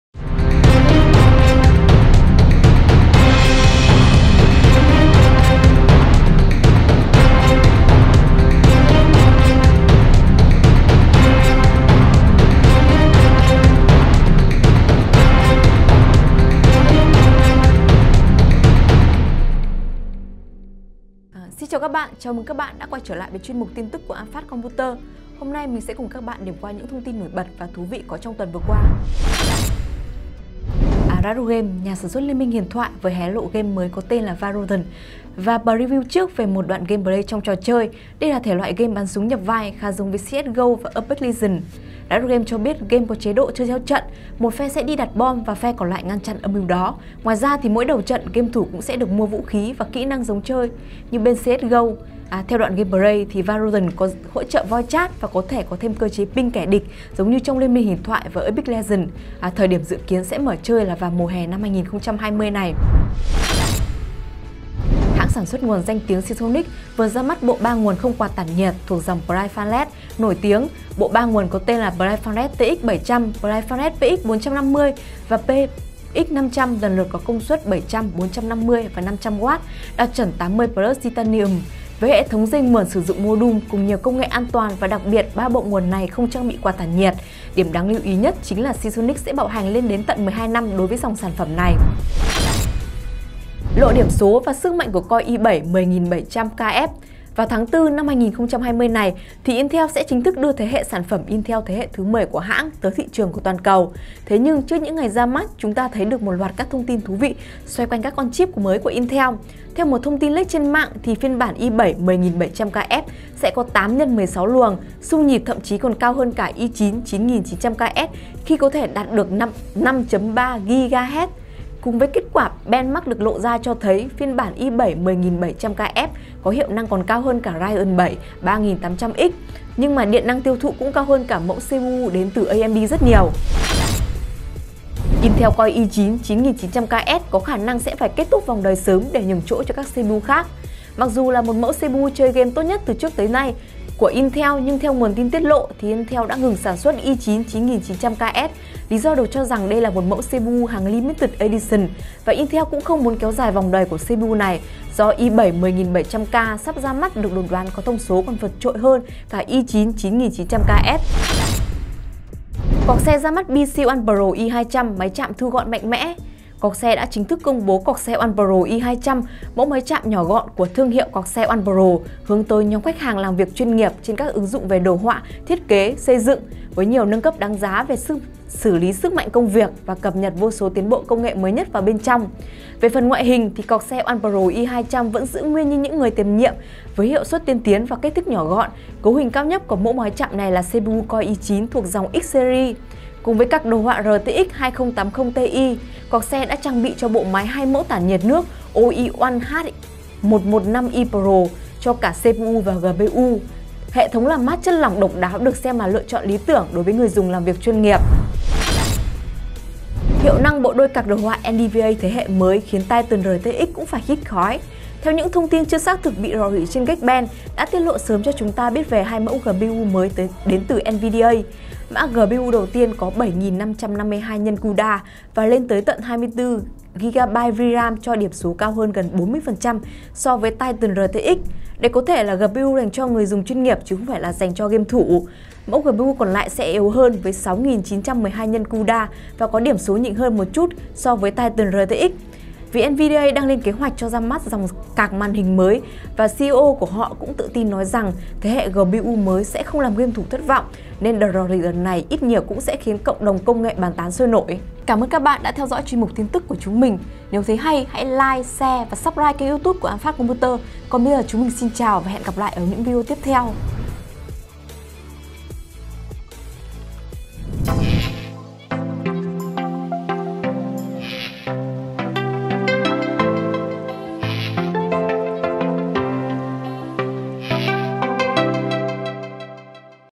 Riot ra mắt tựa Game Valorant - i9 9900KS phải về vườn sớm hơn dự kiến - AP News #2 | An Phat PC
📌Link tư vấn Build PC miễn phí:
Hãy cùng với An Phát chúng mình cập nhật những thông tin hay và hot nhất trong tuần.